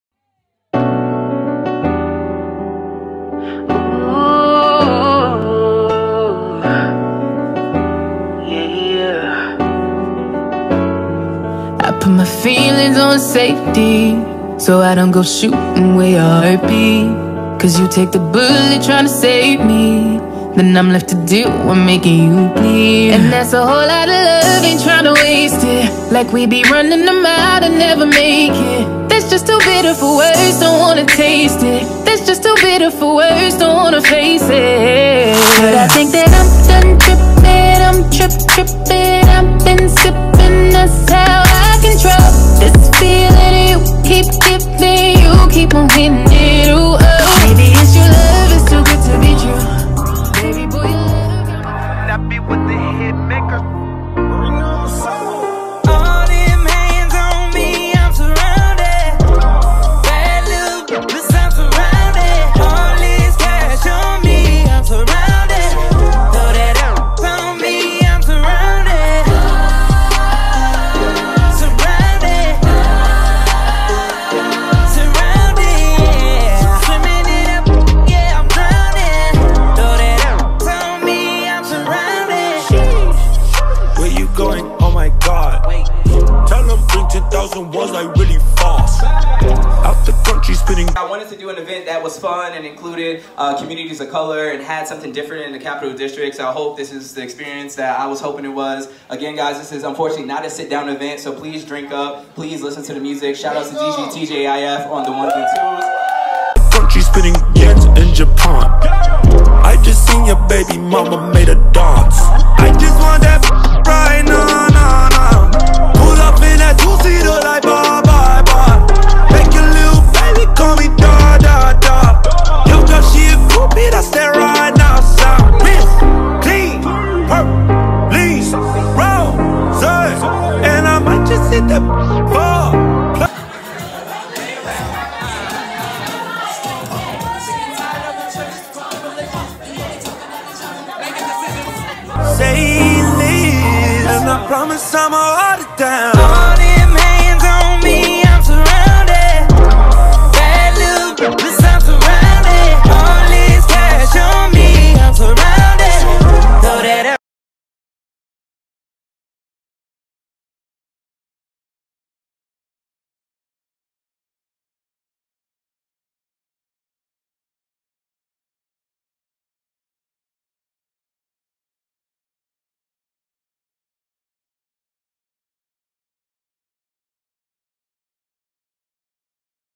8 Count Kitchen R&Brunch Pop-Up
8 Count Kitchen Celebrity Chef Jermaine Wright, from Food Network's All-Star Academy presents his freshman event, R&Brunch Pop-Up at ADCo. This GourmetTurnUp features a custom menu designed and developed by Chef Jermaine Wright and includes

-3 HRS OF BOTTOMLESS MIMOSAS
-CHEFS CHOICE GOURMET BRUNCH BUFFET
-SIGNATURE COCKTAILS
-LIVE CELEBRITY CHEF COOKING EXPERIENCE
-LIVE DJ
-BRUNCH PARTY ADMISSION

Enjoy all of your favorite new school & old school reggae, hip-hop, and Afro-Latin hits, with an emphasis on R&B and Neo Soul, because if you didn't know,

Filmed by: Vanessa "Diddy" Cole @rackzondiddy
Edited by: Vanessa "Diddy" Cole @rackzondiddy
Executive Chef: Jermaine Wright
Sous Chef: Brandon Lee